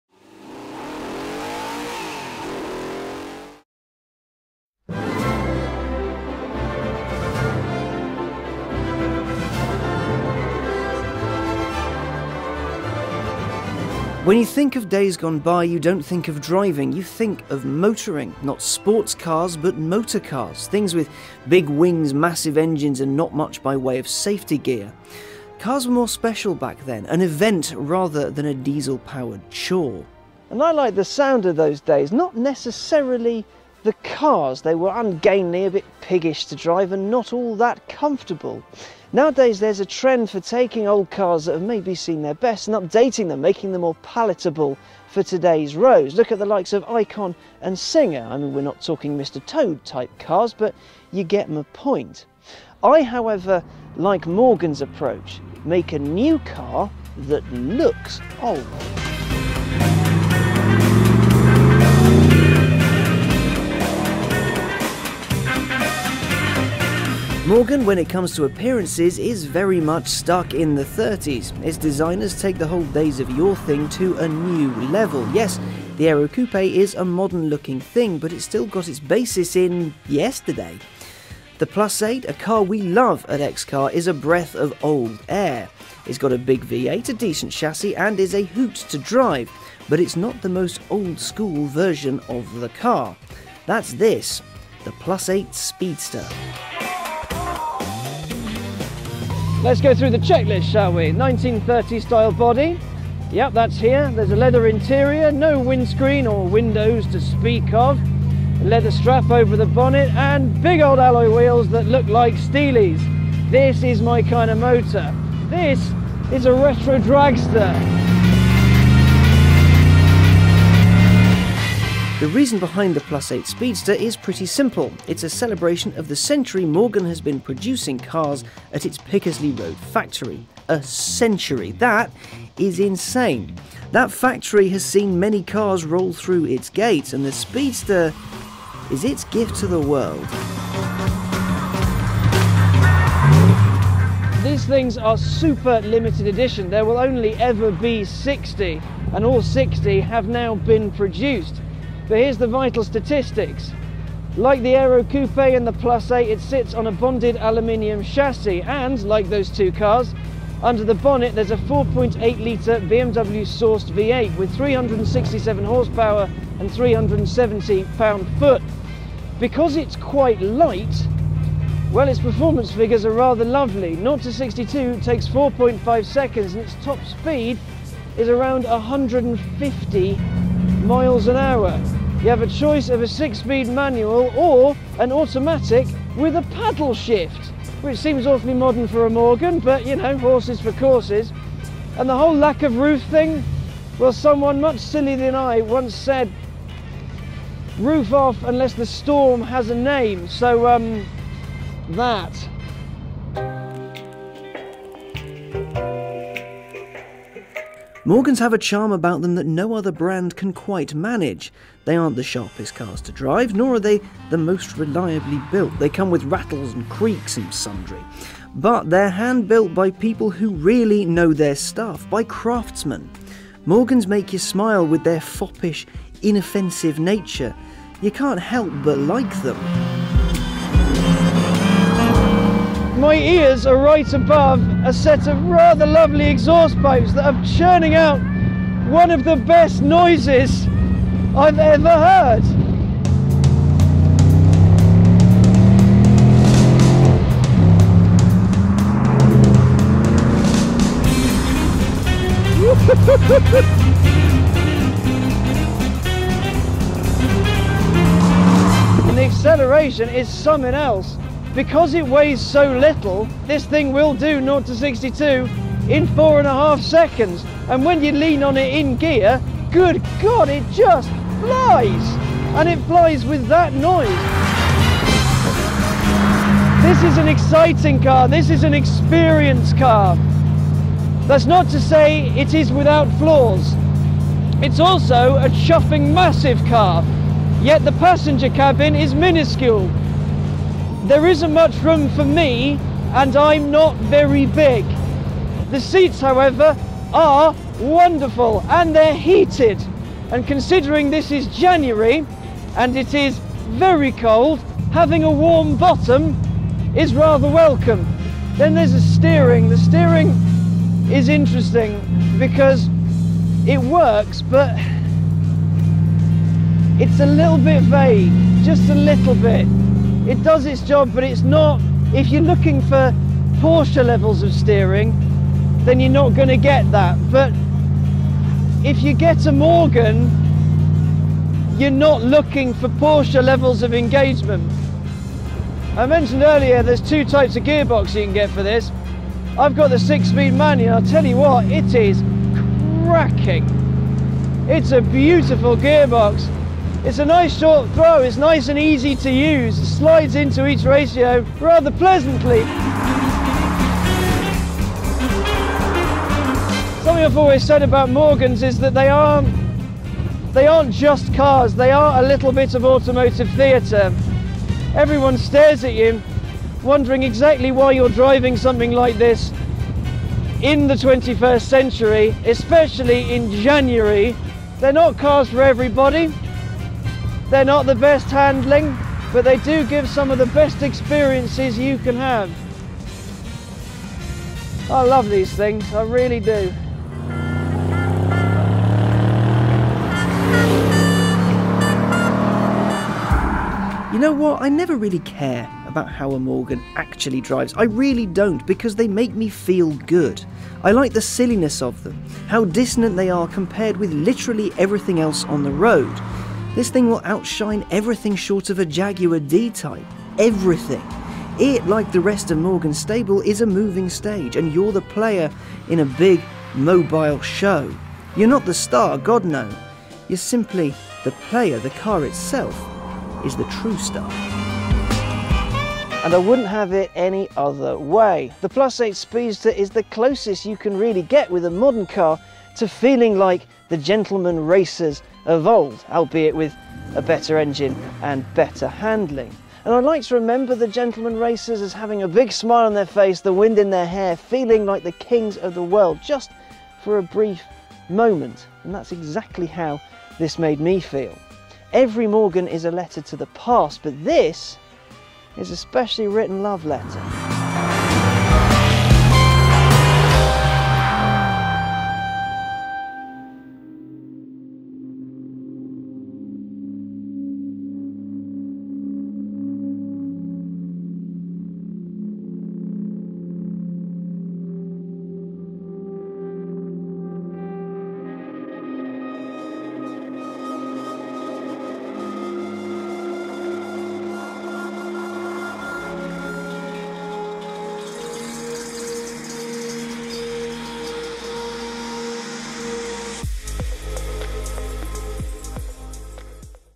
Morgan Plus 8 Speedster: Looks Retro, Sounds Like Thunder - XCAR
Morgan looks to the past with more than a sense of nostalgia, it's their source of inspiration. We drive the limited edition Plus 8 Speedster.

Join the XCAR community...

VIDEOS IN ENDBOARD